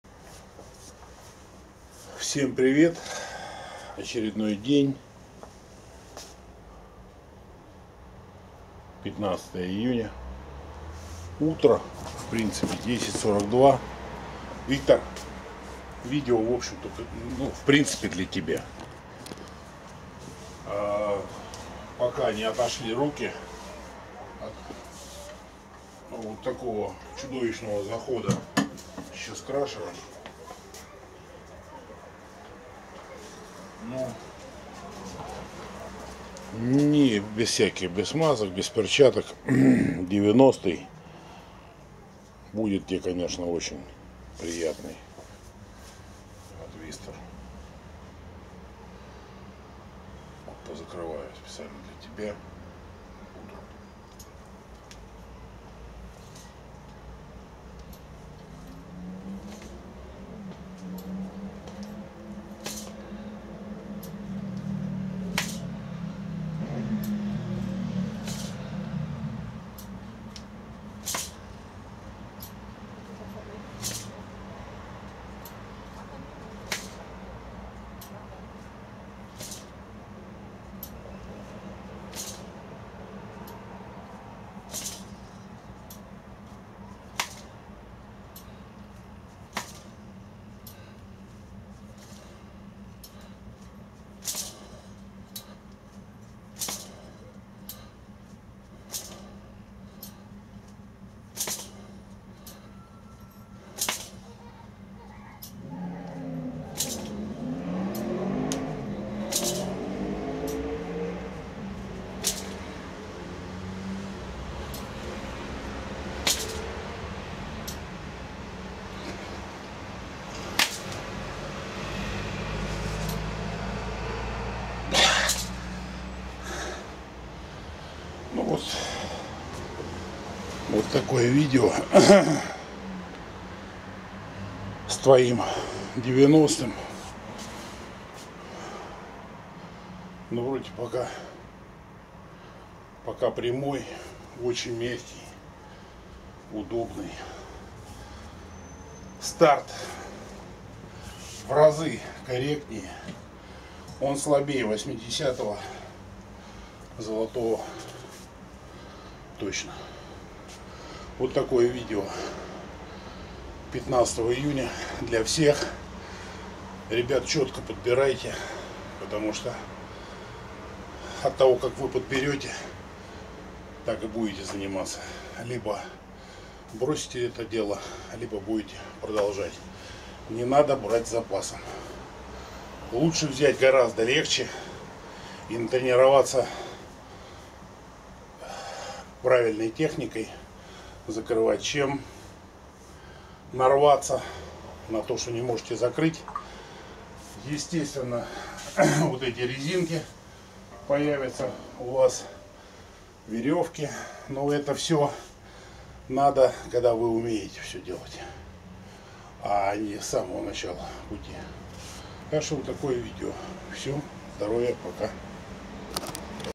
Персональный рекорд с твистером Haoying Black 90kg на 24 закрытия
Точно можно гораздо больше,но не нужно.
Закрыл 24 раза за 60 сек.Темп 2,5 сек.экстремальный очень.Снижение темпа до 5-7сек.на одно закрытие увеличит минимум в два раза количество закрытий по итогу,что и произошло с золотым 80кг.А так 90кг чёрный это 80кг золотой по факту.